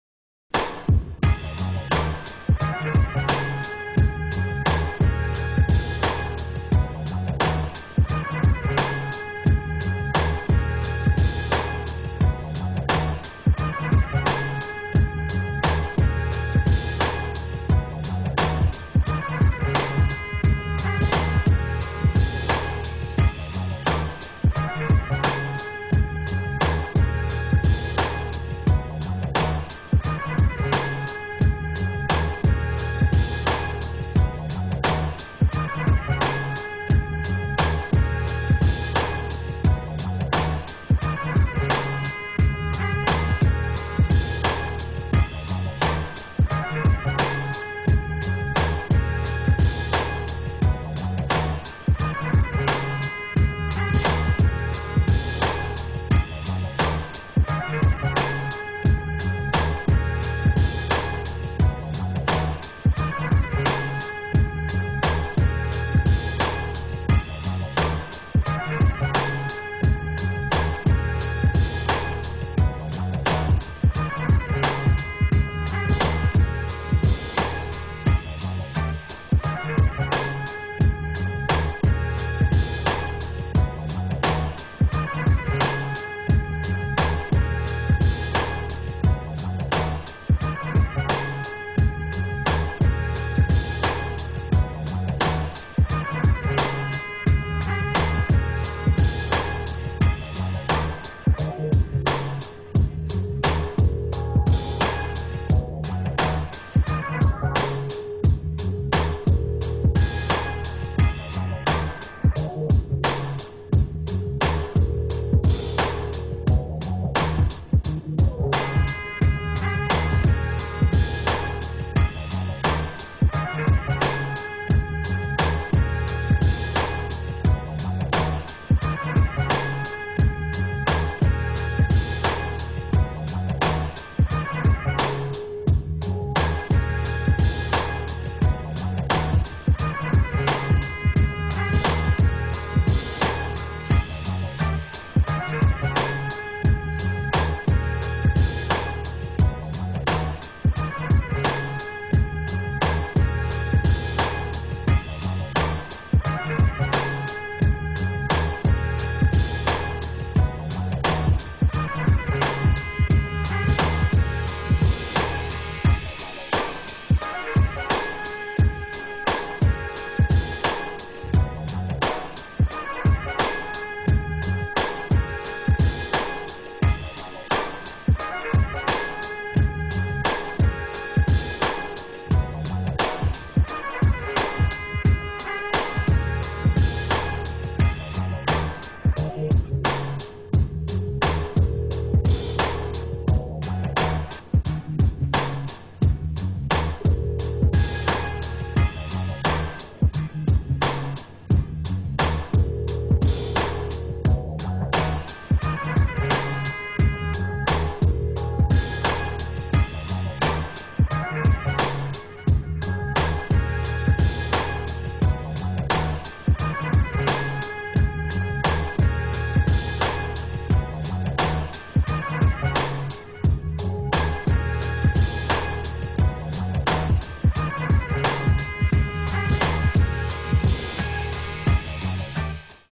Jehst YNR
Video 60 from Writers Block, presenting the High Plains Drifter aka Jehst YNR.